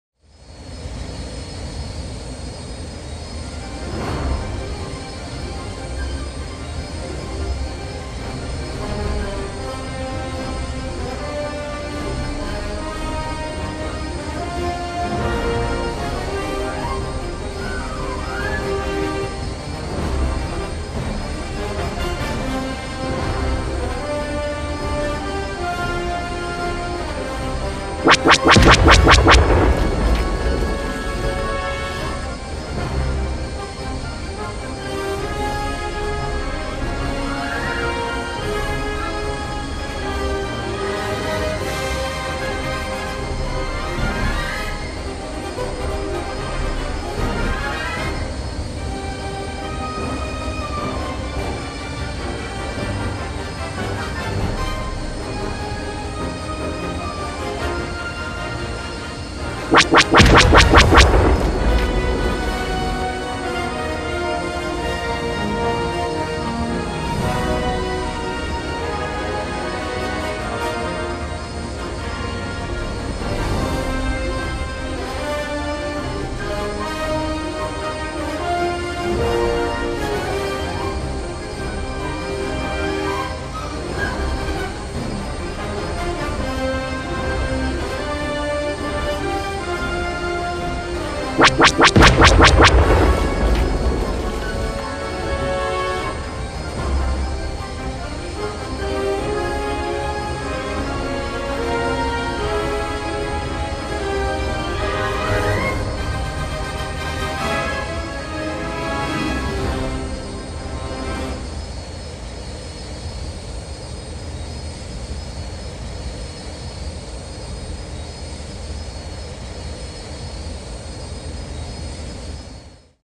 X-Wing Chase on Tatooine desert Wallpaper (Obi-Wan)
X-Wing Chase on Tatooine desert , a wallpaper made for the special occasion of the launch of the Obi Wan Kenobi series !

The Wallpaper name is: X-Wing Chase on Tatooine desert (Obi-Wan)